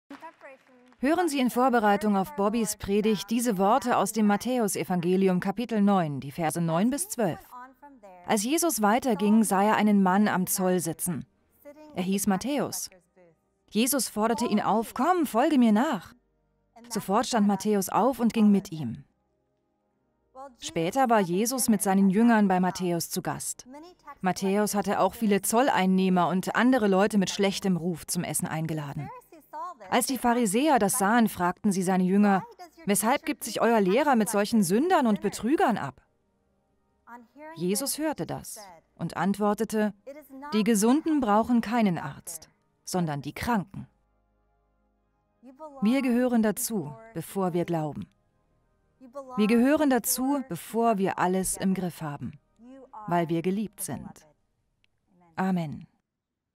Bibellesung von Hannah Schuller: Matthäus 9, 9 bis 12
Hören Sie in Vorbereitung auf die Predigt "Du bist geliebt" von Bobby Schuller Worte Gottes aus dem Matthäusevangelium Kapitel 9, die Verse 9 bis 12. Wir gehören dazu, bevor wir glauben. Wir gehören dazu, bevor wir alles im Griff haben. Weil wir geliebt sind. Amen.